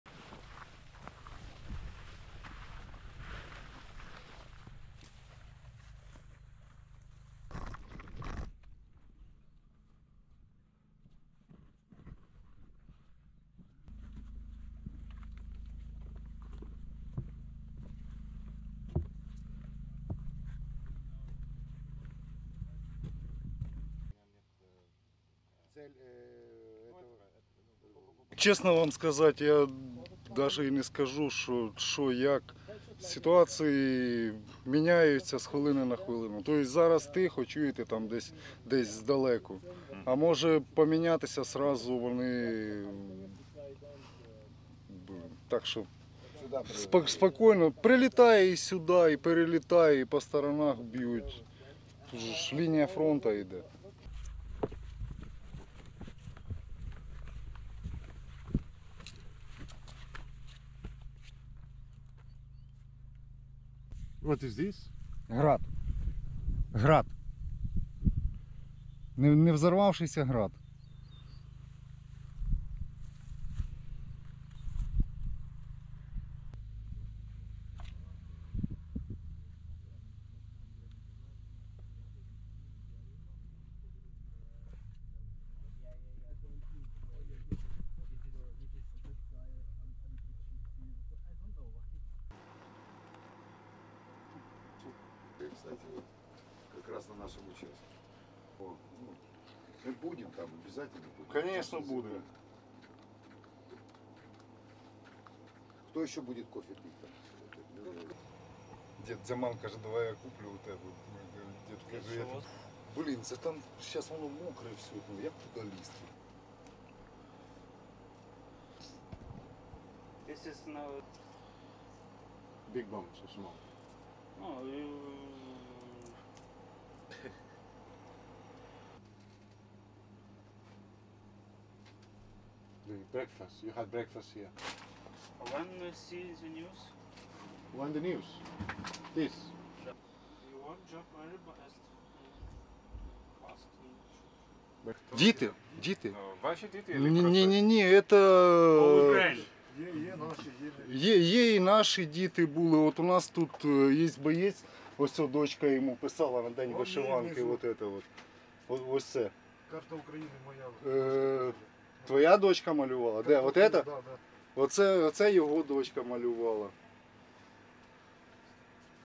Миколаїв | Військові ЗСУ на лінії фронту
Війська РФ регулярно обстрілюють Миколаївську область, зокрема використовуючи заборонені касетні боєприпаси. Проте військовим Збройних Сил України вдається тримати оборону та дають відсіч противнику. Як живуть військові на лінії фронту та що кажуть про ситуацію на цьому напрямку – дивіться у сюжеті.